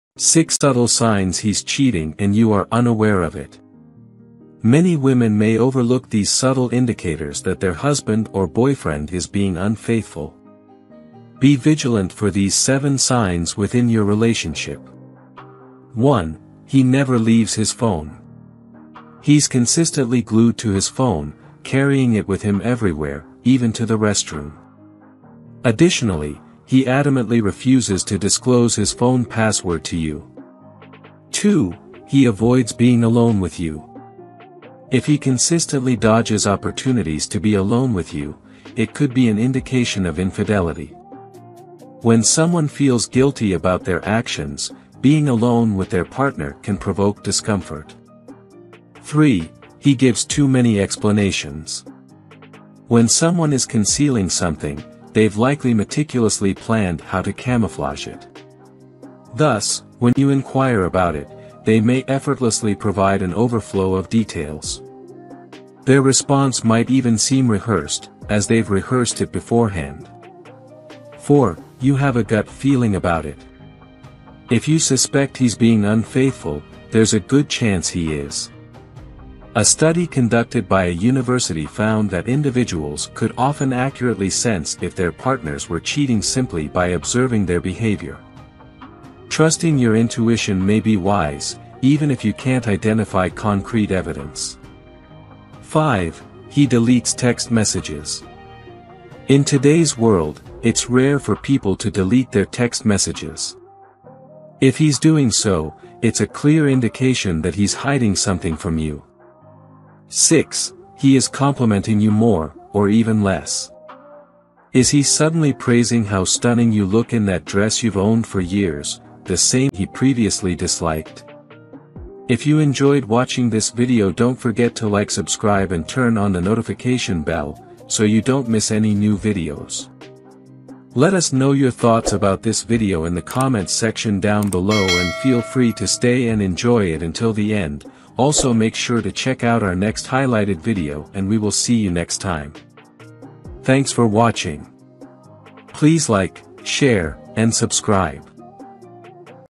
6 Subtle Signs He's Cheating And You Are Unaware Of It | Human Behaviour Psychology | Awesome Facts
6 Subtle Signs He's Cheating And You Are Unaware Of It | Human Behaviour Psychology | Awesome Facts
Many women may overlook these subtle indicators that their husband or boyfriend is being unfaithful. Be vigilant for these seven signs within your relationship.
1. He never leaves his phone.
2. He avoids being alone with you.
3. He gives too many explanations.
4. You have a gut feeling about it.
5. He deletes text messages.
6. He is complimenting you more (or even less).